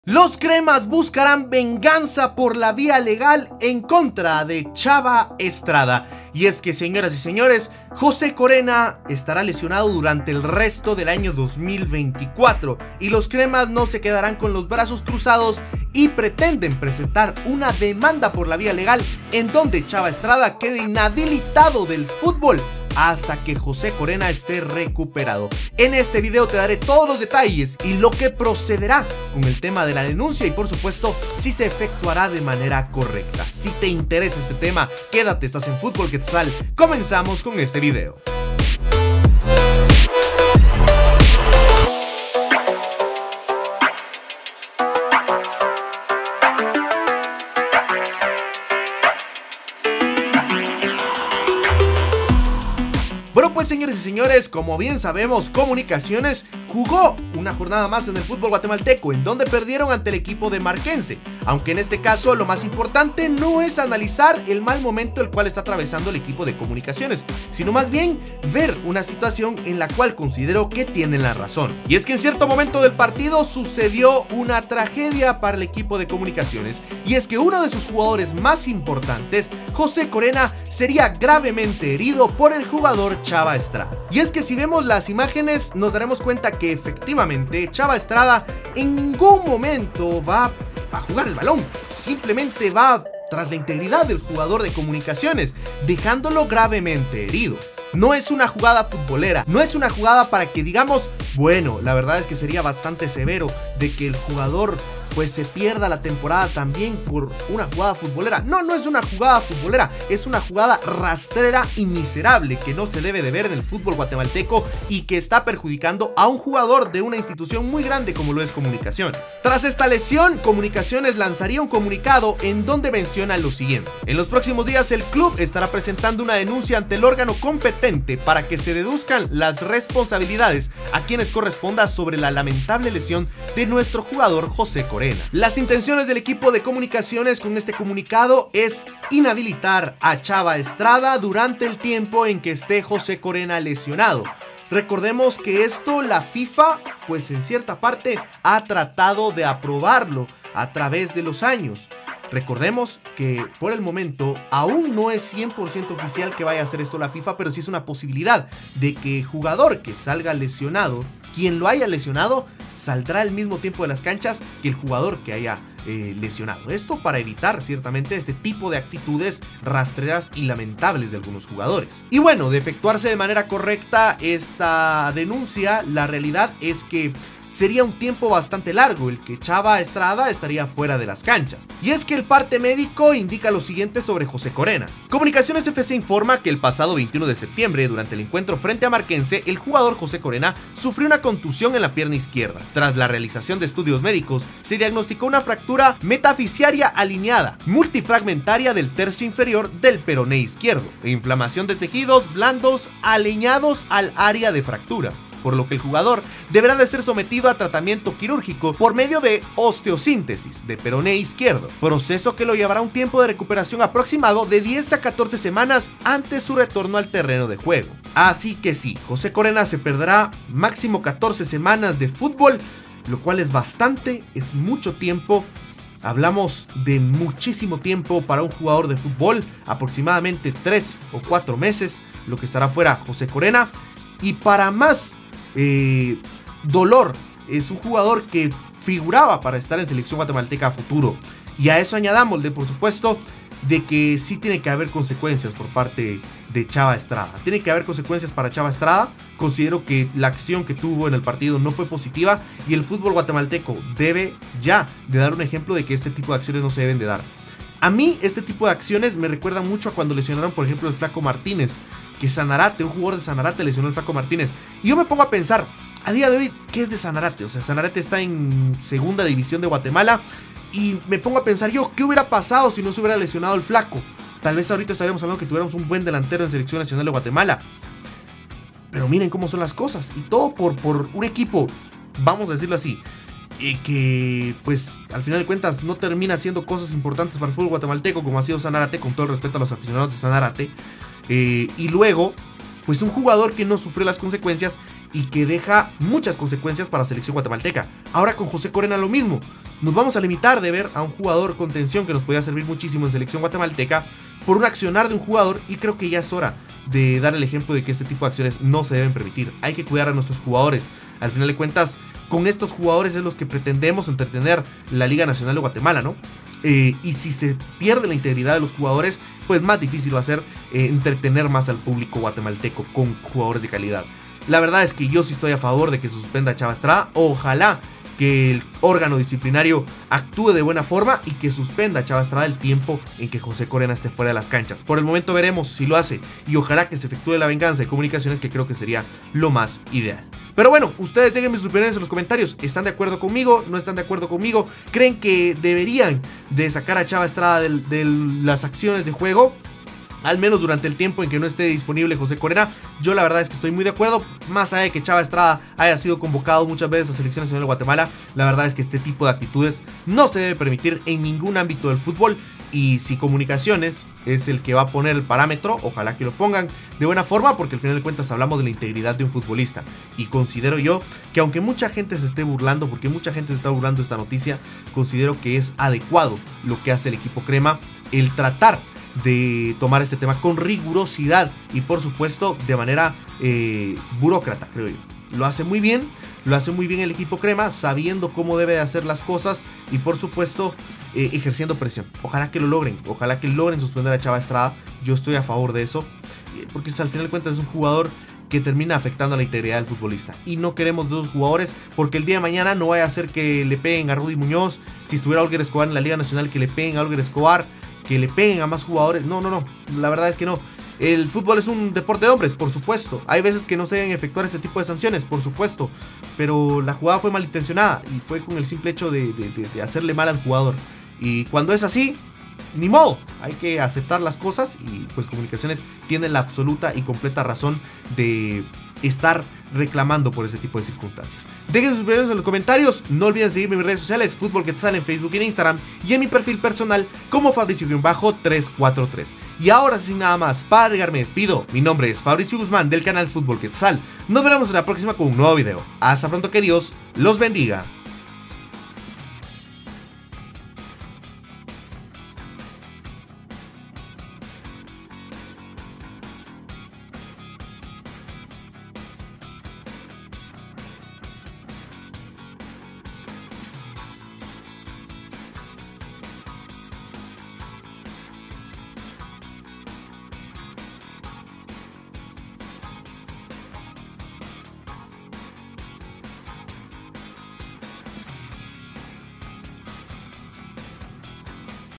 LOS CREMAS buscarán VENGANZA por la Vía LEGAL | Comunicaciones denunciará a Chava Estrada
¿𝙄𝙙𝙚𝙖𝙨, 𝙨𝙪𝙜𝙚𝙧𝙚𝙣𝙘𝙞𝙖𝙨, 𝙤 𝙨𝙞𝙢𝙥𝙡𝙚𝙢𝙚𝙣𝙩𝙚 𝙦𝙪𝙞𝙚𝙧𝙚𝙨 𝙘𝙝𝙞𝙨𝙢𝙤𝙨𝙚𝙖𝙧?

¡Gracias por estar pendientes y nos vemos en el siguiente videazo! 🚀⚽ Mi nombre es Fabrizio Guzmán ¡Hasta pronto, cracks!